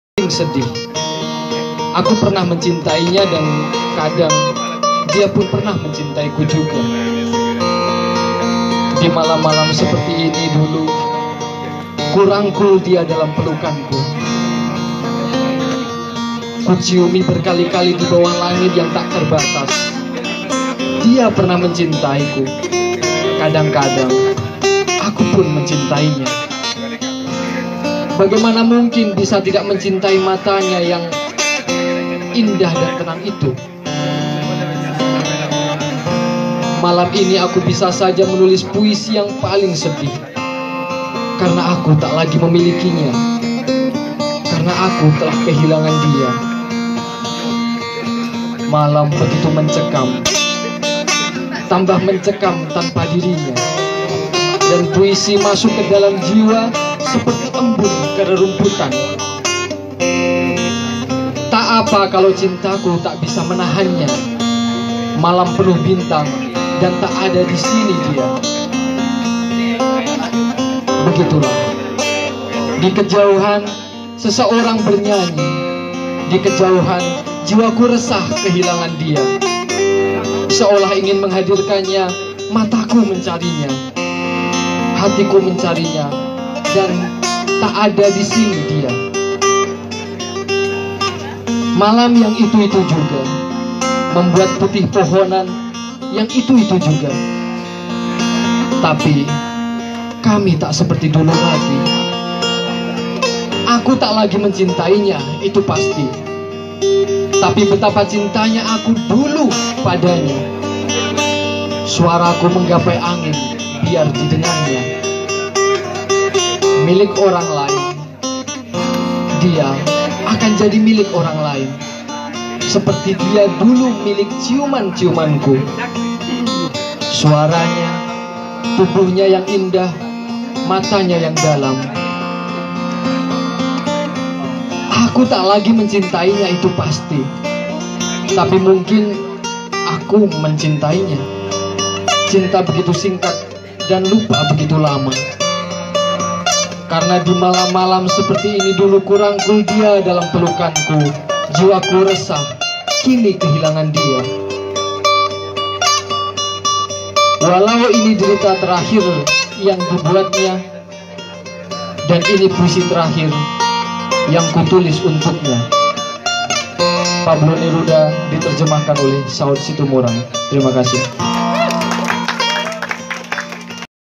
Alfin Rizal Membaca Puisi Pablo Neruda - APSAS #11 Apresiasi Sastra Radio Buku Yogyakarta
Video oleh: Adam Radjulan
Alfin Rizal berkolaborasi dengan pemain Sape dari Kalimantan Barat.